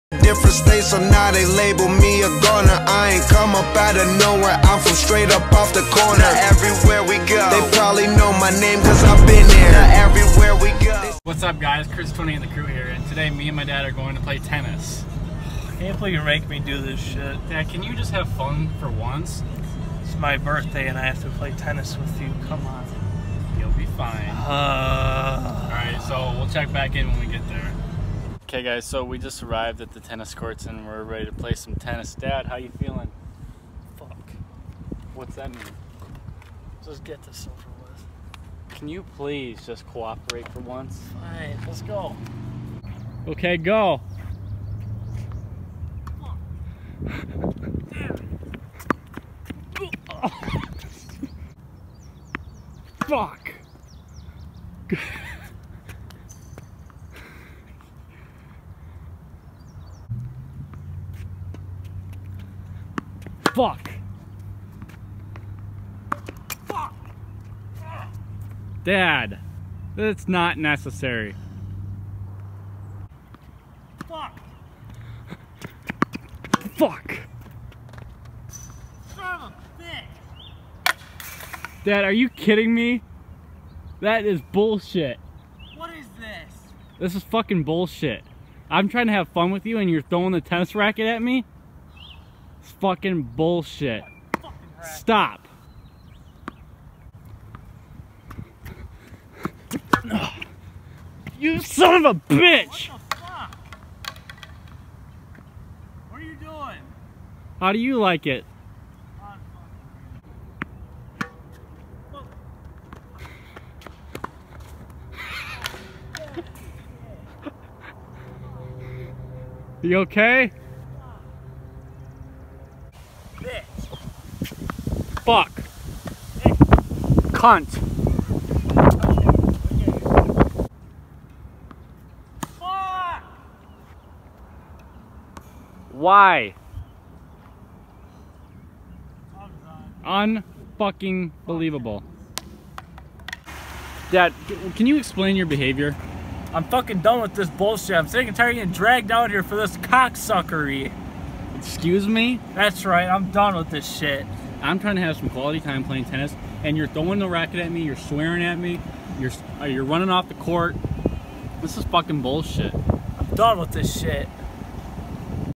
Psycho Dad’s Tennis Freakout!
Psycho has a meltdown while playing tennis.